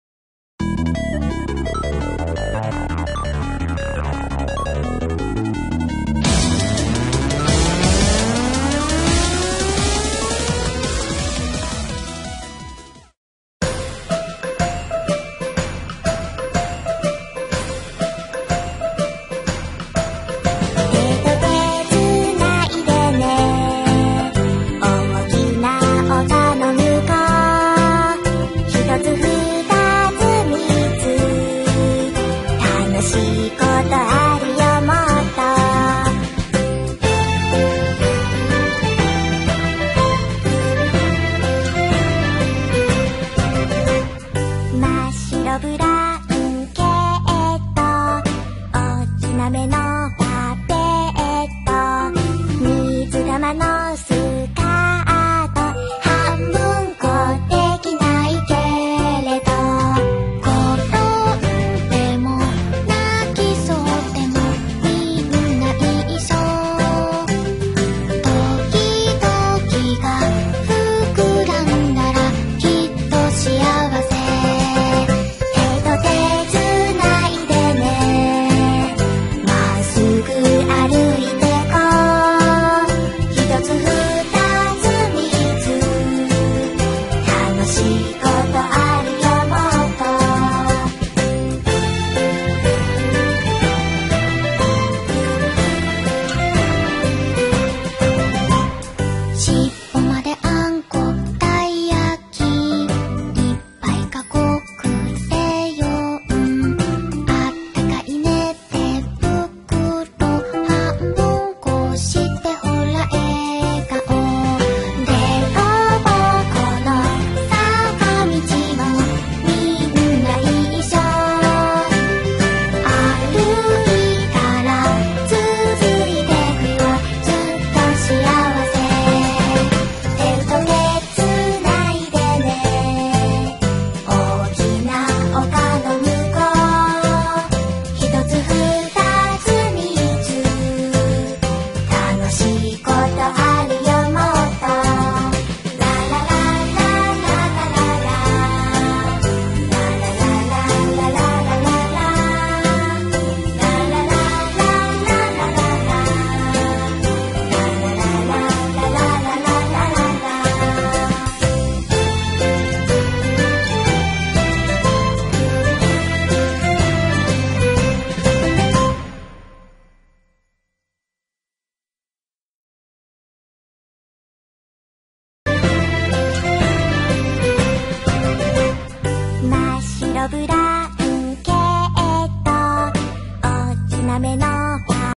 SpeedART Wing
I made my own Oc Wing :D And in this video I tried some new stuff :P Enjoy!